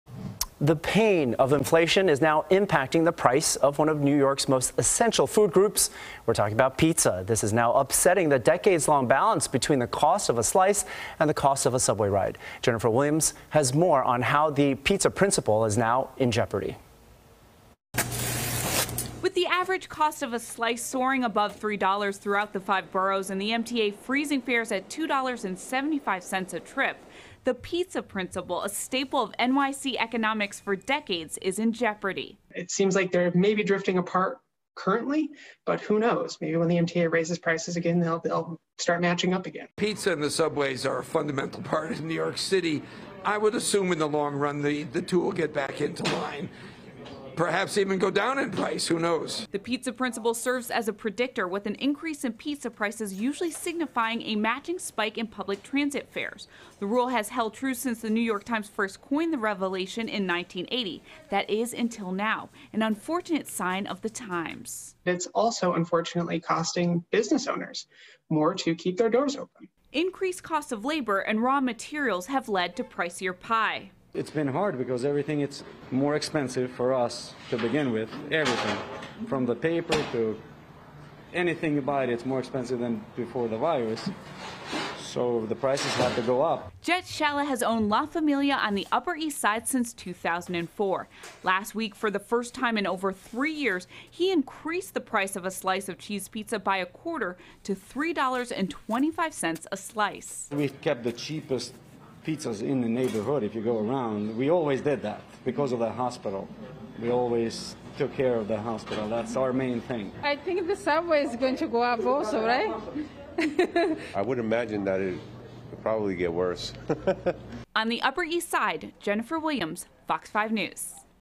NYC pizza principle in jeopardy
The pain of inflation is now affecting the price of one of New York City's most important food groups, pizza. The decades-long balance between the cost of a regular slice and the cost of a subway ride is in jeopardy.